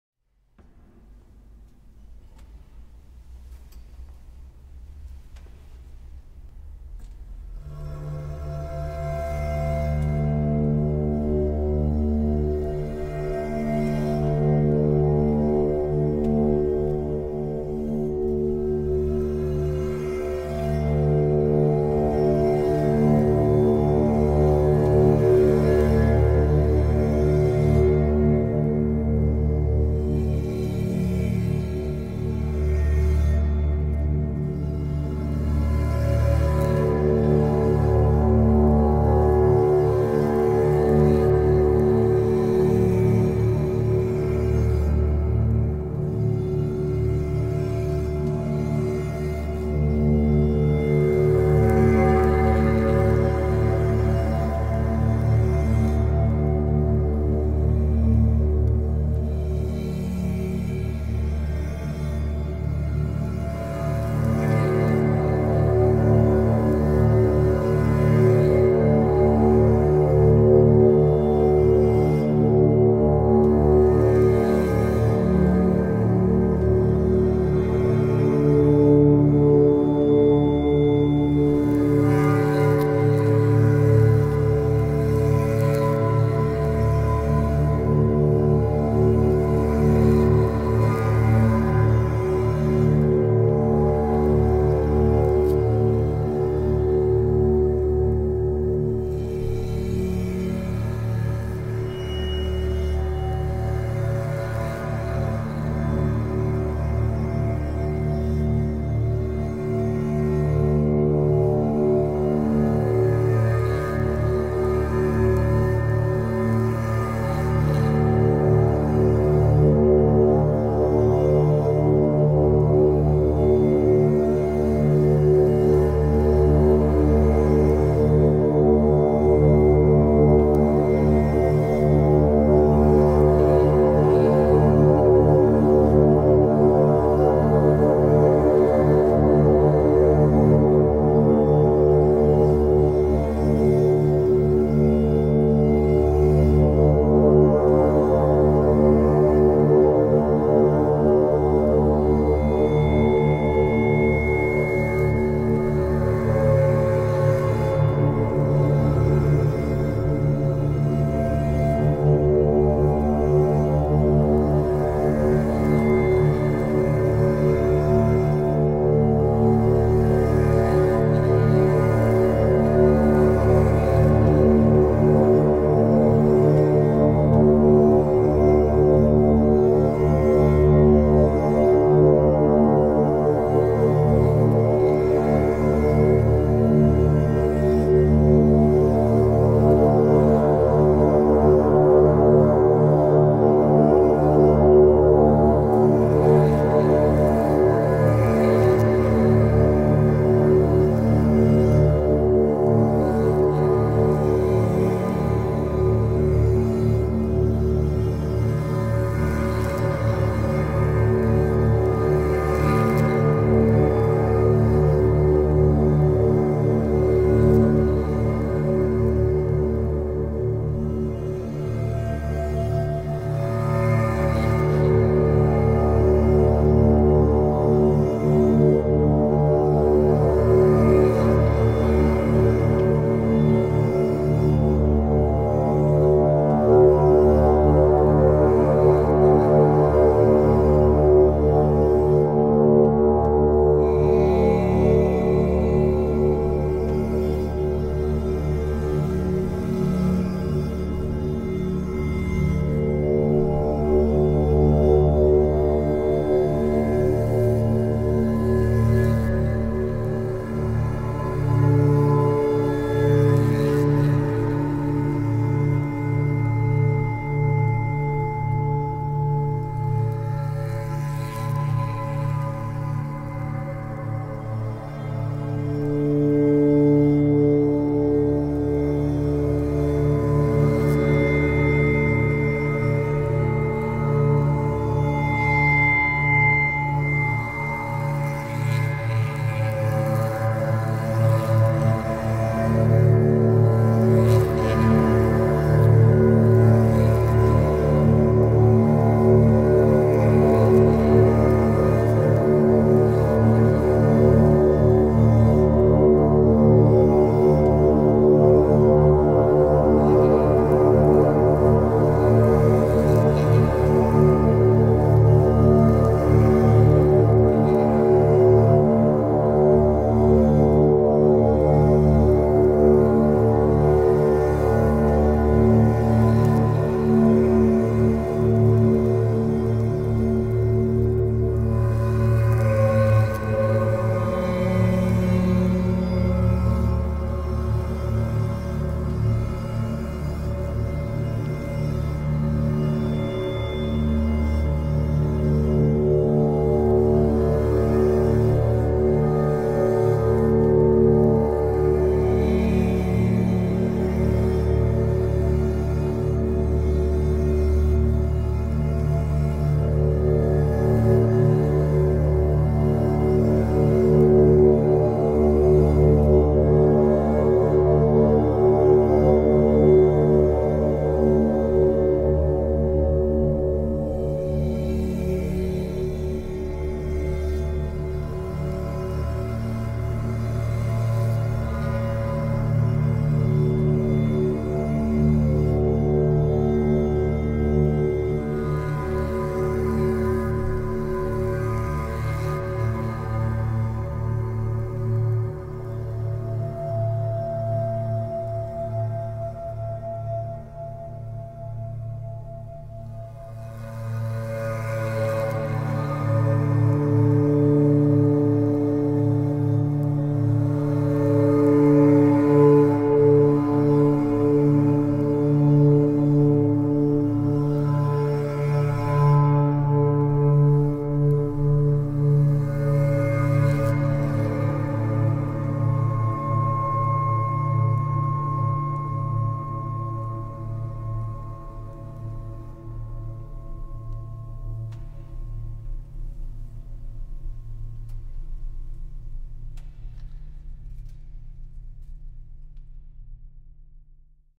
Wóma
Wóma is the alias of English composer Arran Poole. As Wóma, Arran is particularly interested in ‘primeval’ sound; sound that’s evocative of primitive artistic practices and languages, and that largely disregards the established rules of contemporary music.

Arran’s instrument of choice is an unusual noise-making device; a part-instrument, part-sculpture called a ‘bow chime’. He adopted this instrument around 15 years ago after seeing a performance by its inventor, Robert Rutman and other practitioners in Coventry. The bow chime itself comprises a large stainless steel sheet, attached to which are a series of tuned rods. When bowed, the instrument produces deep, thunderous resonances and guttural, growly utterances.

This video is a short drone work produced in Arran’s studio in Essex, solely featuring two layers of bow chime improvisation.